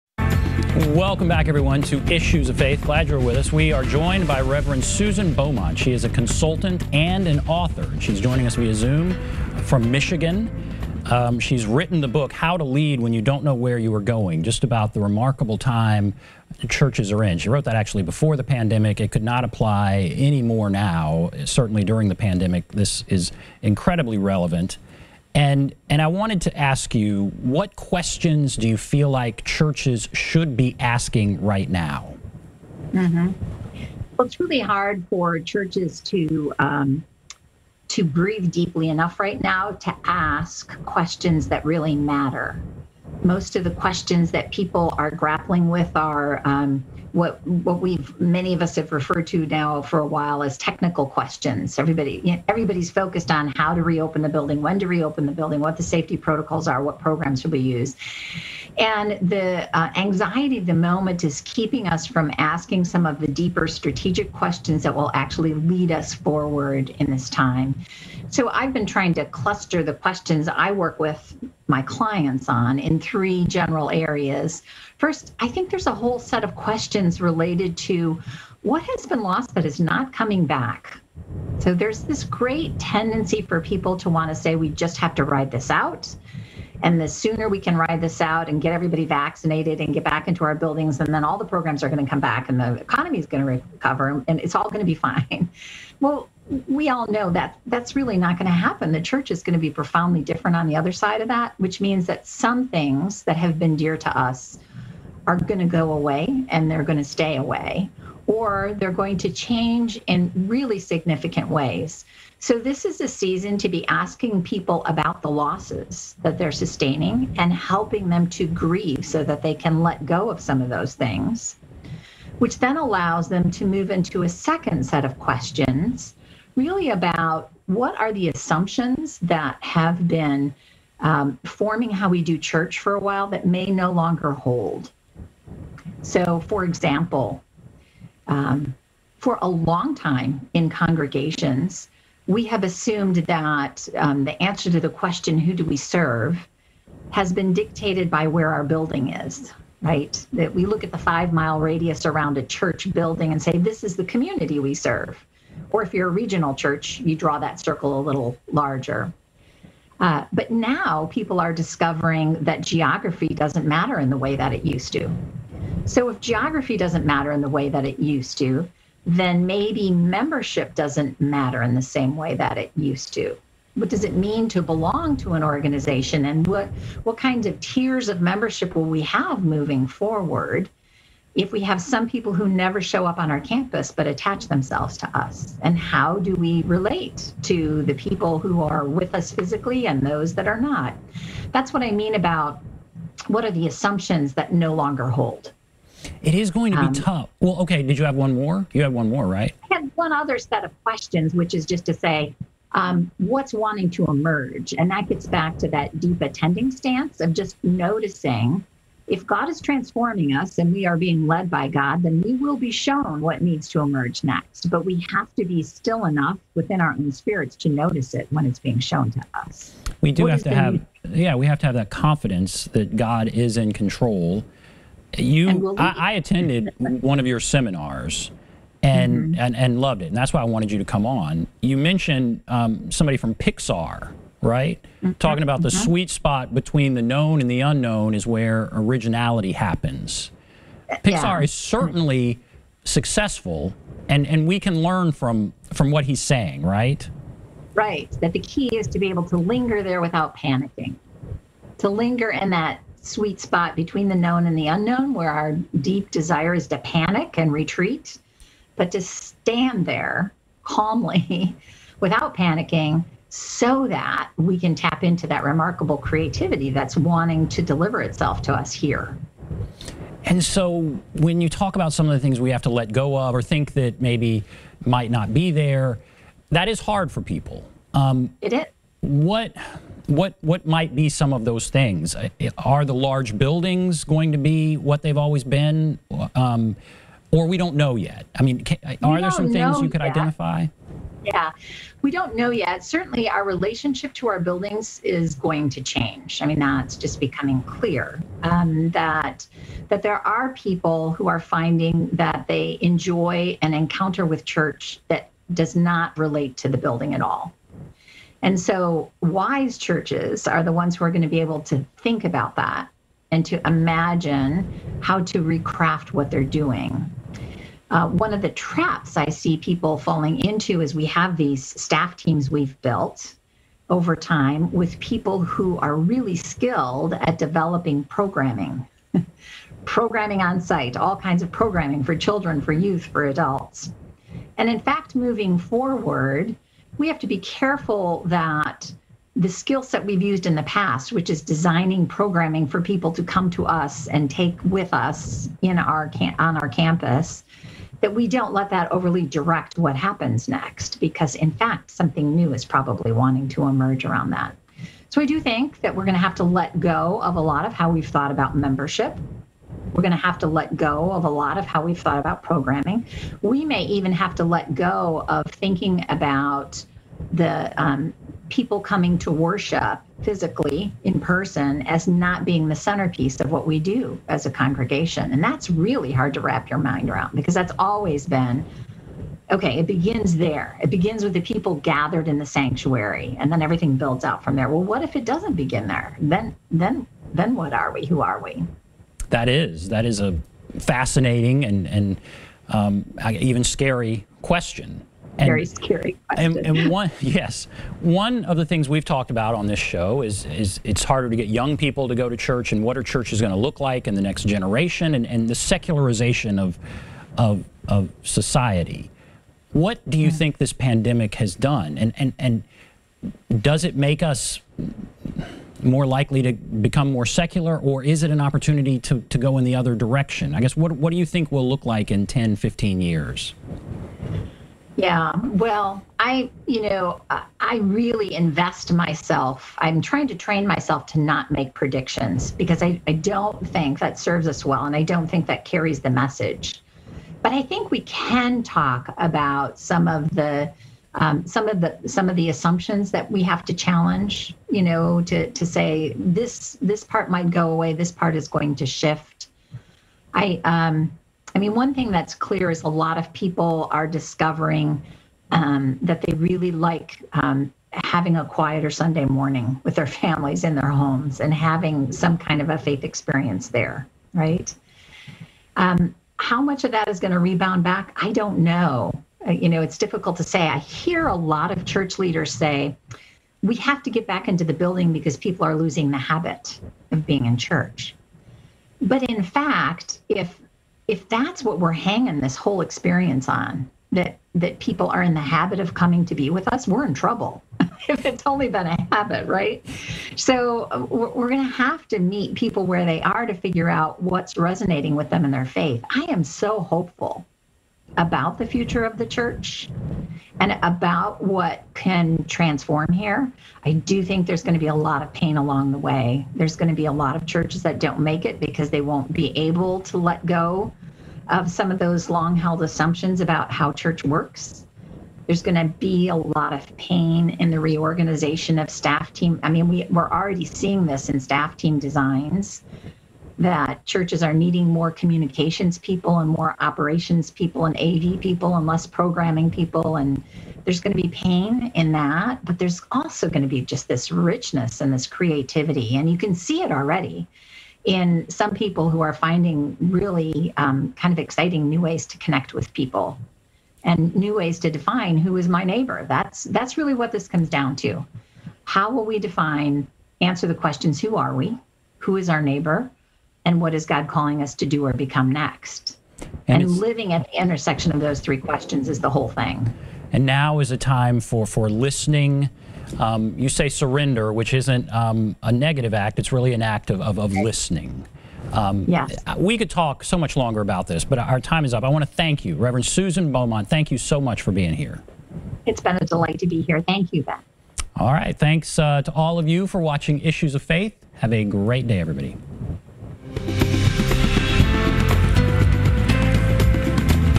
Issues of Faith: Church leadership in uncertain times p2
Susan Beaumont, author of "How to Lead When You Don't Know Where You're Gong" joins Ben Hall to discuss efforts church leadership must consider in these uncertain times.